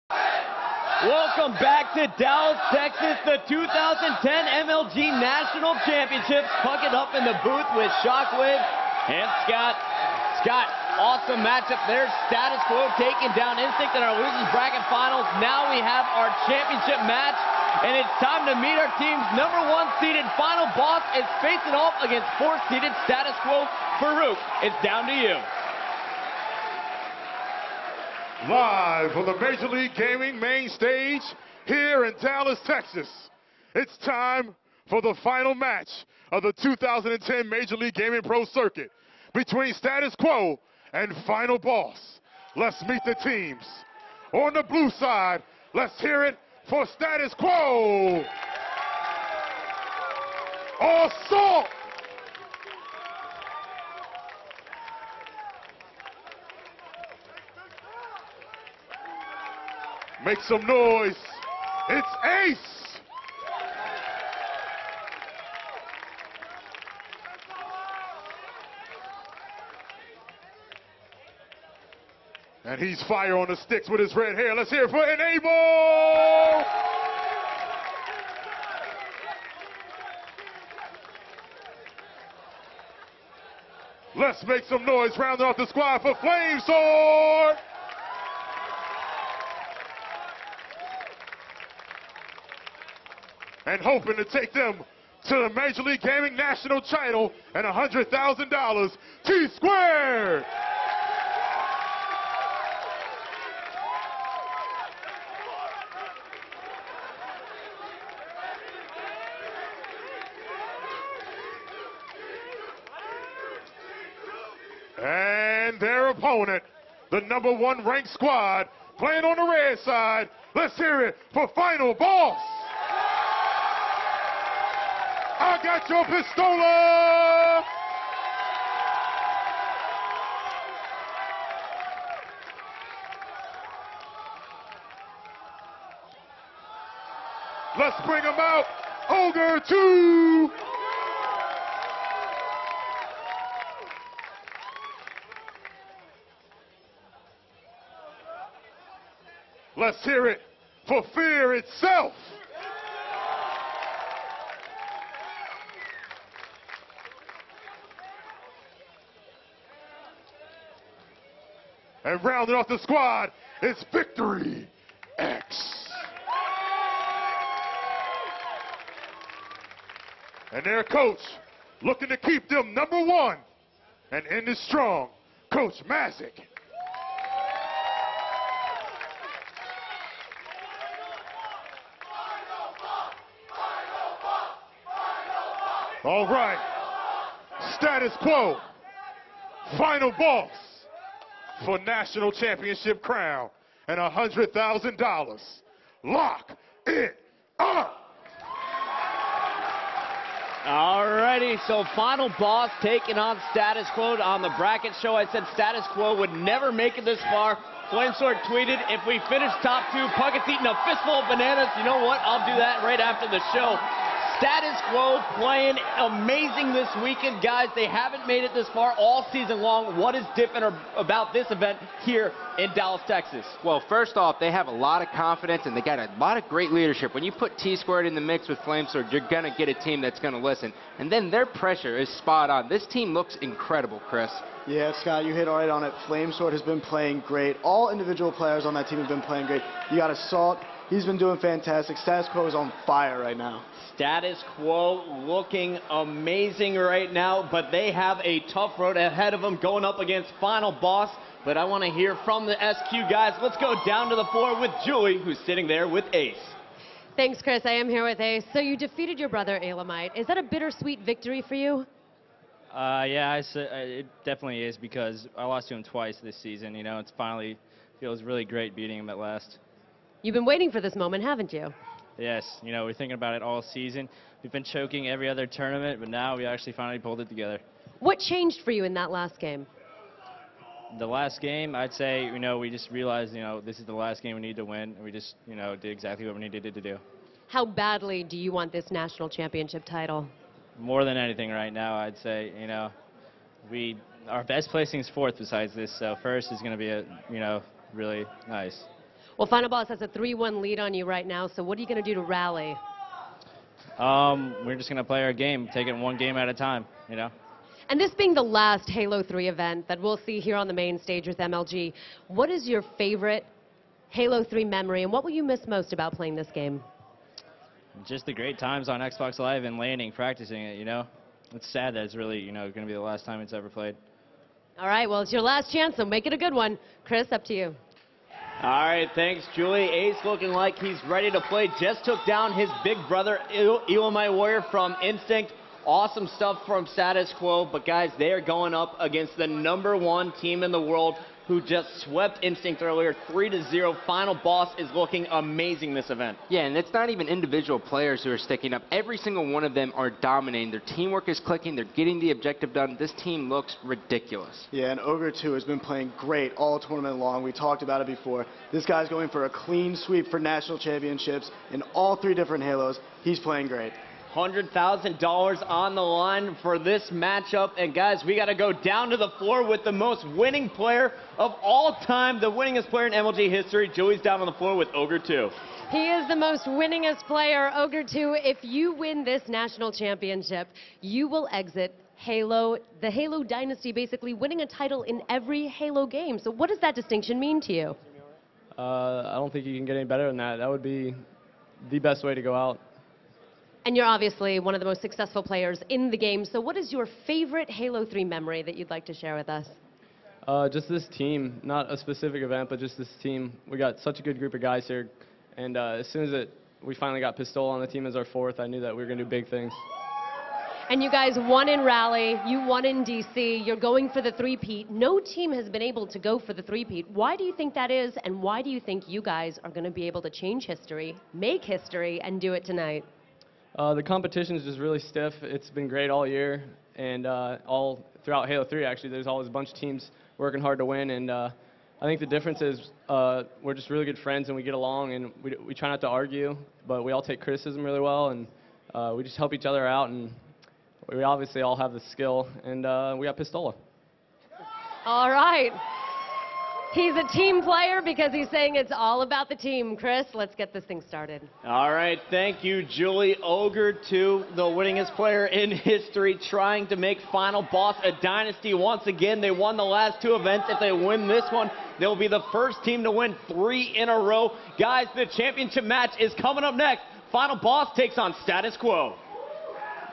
Dallas - Finals - Final Boss vs. Status Quo (Part 1) - MLG Casting
MLG 2010 - Dallas - National Championships
Final Boss vs. Status Quo
MLG casting